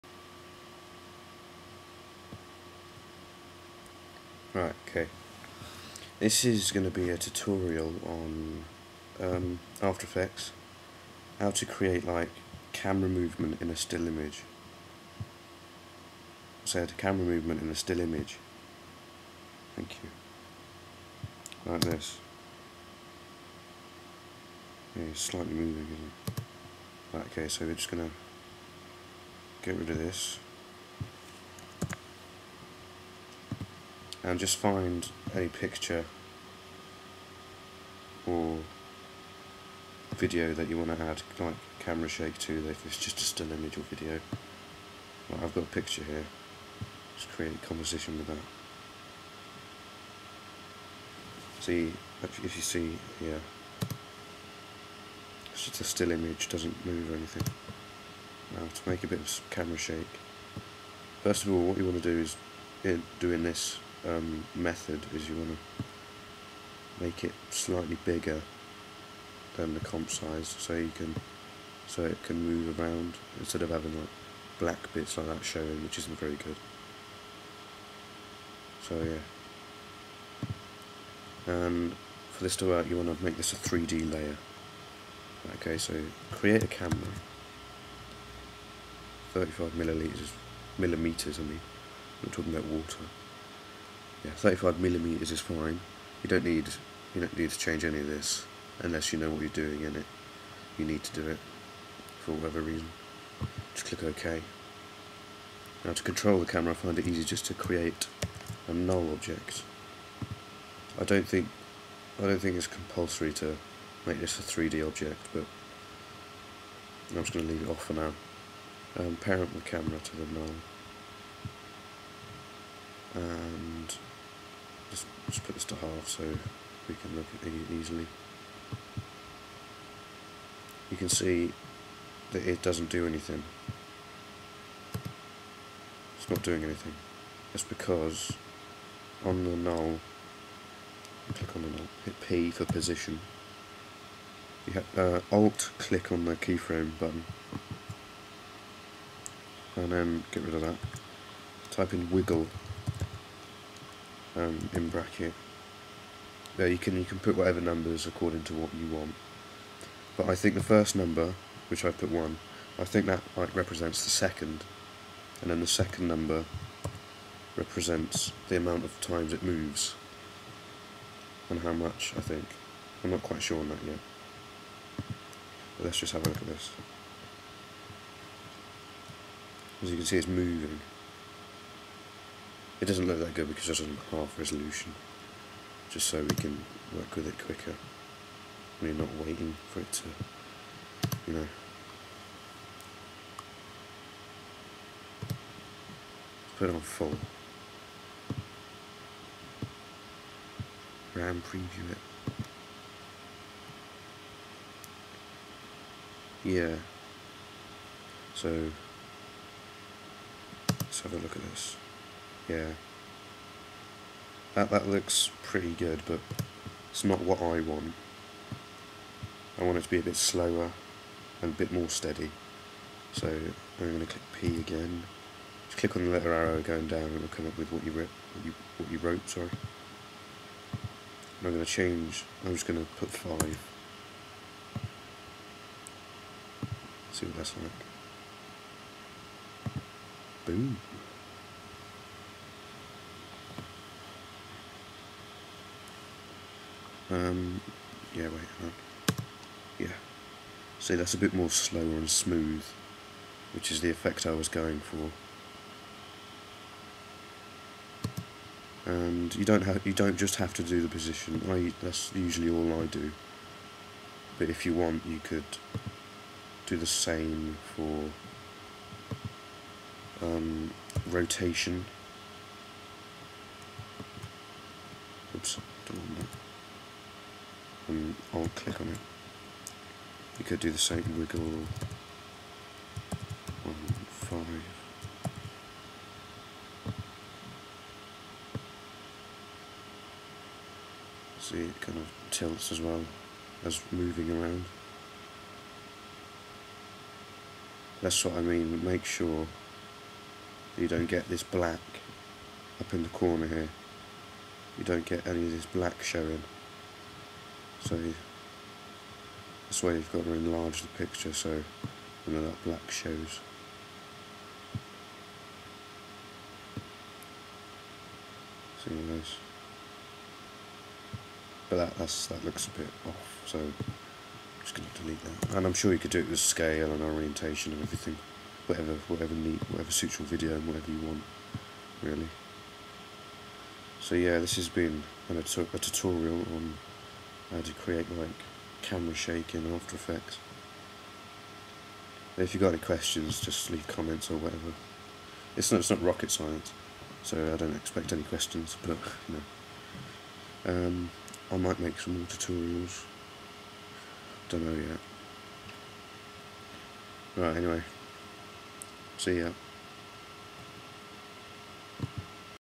After Effects Tutorial: Camera shake on still footage.
this is my FIRST tutorial so bare with me. This is a basic Tut on how to create some camera shake on a still picture or video clip using the built in 'wiggle' in After Effects,

NEEDED:
Still Video clip or Picture.

Any questions or anything PM me or leave a comment :).